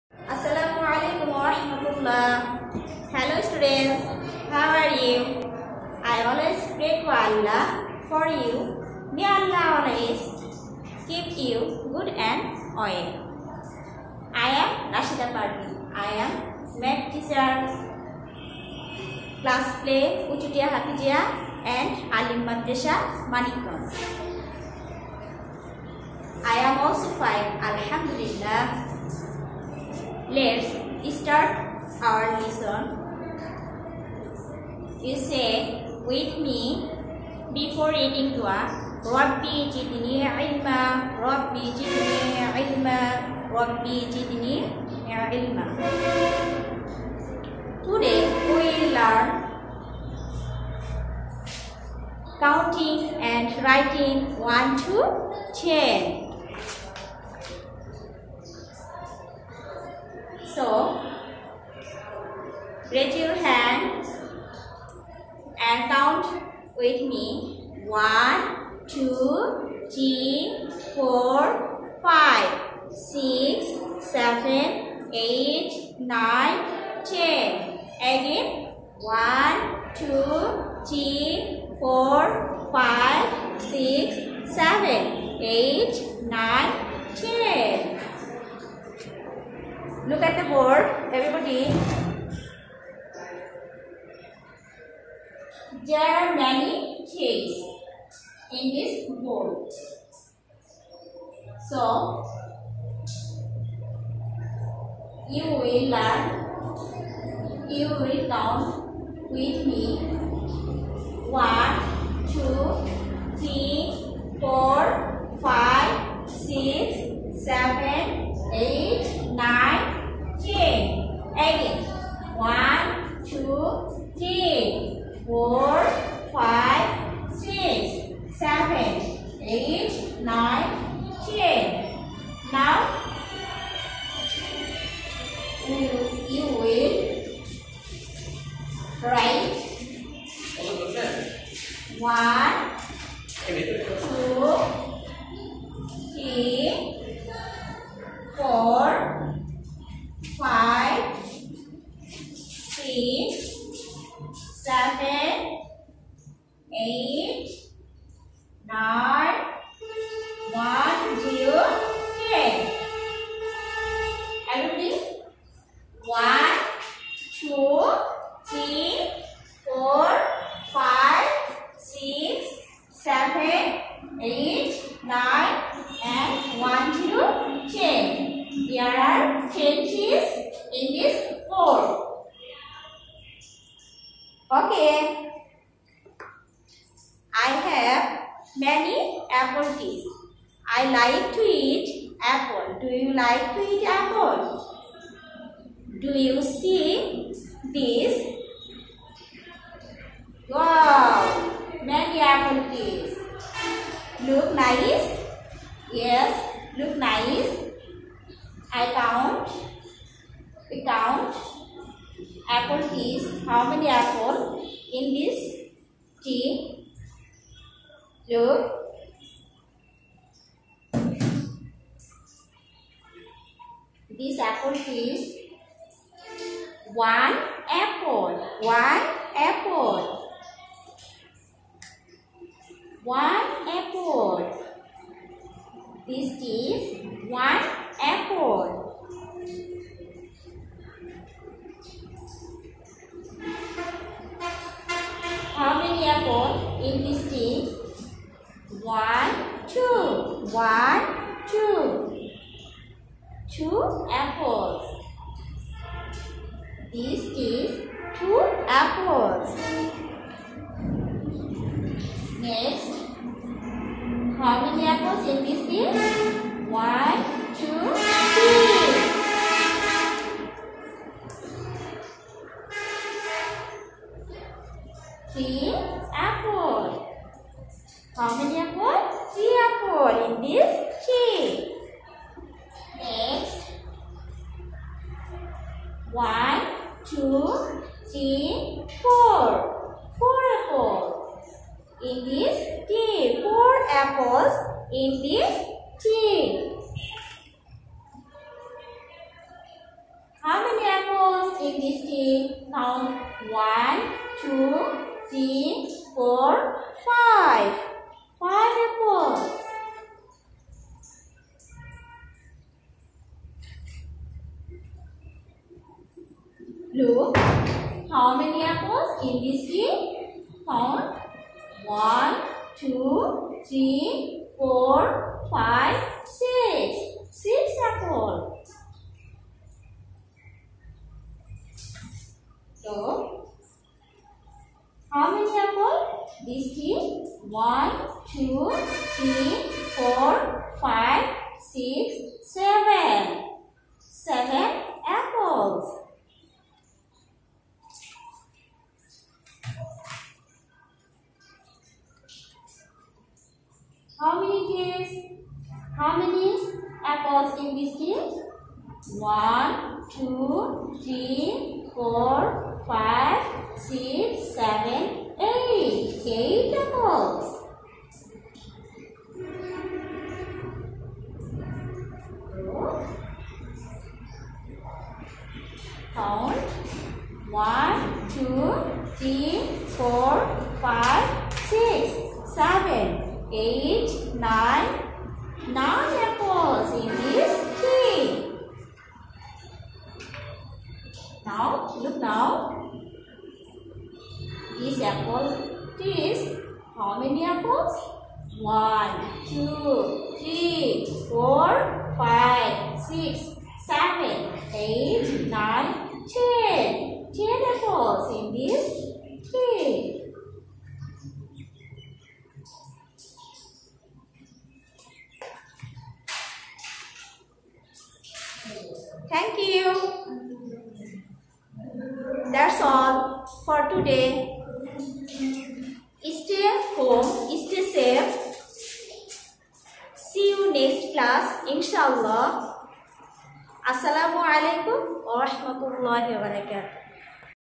Class play math. উচু‌টিয়া হা‌ফি‌জিয়া ও আ‌লিম মাদ্রাসা।
Teachets name Rasheda parvin
 উচু‌টিয়া হা‌ফি‌জিয়া ও আ‌লিম  মাদ্রাসা।